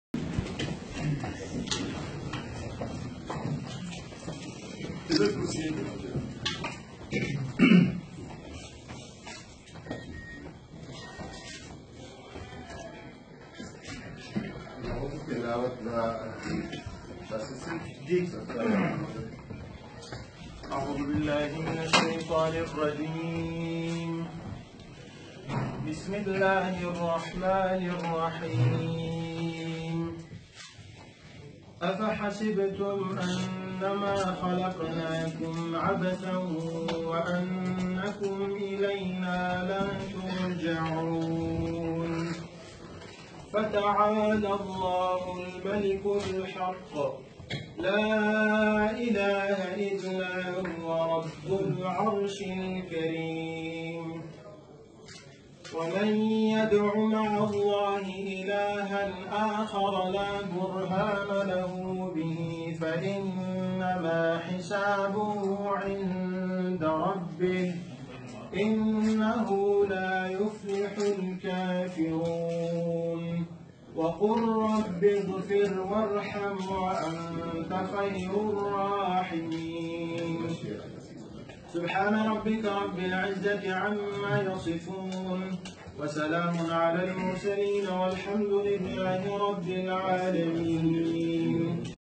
PTI Imran Kha And Dr Noorulhaq AlQadri in PTI Shamuliati Jalsa Iftitah Tilawat e Quran in Banigala
PTI shamuliati Jalsa Dr Noorulhaq Qadri With Imran khan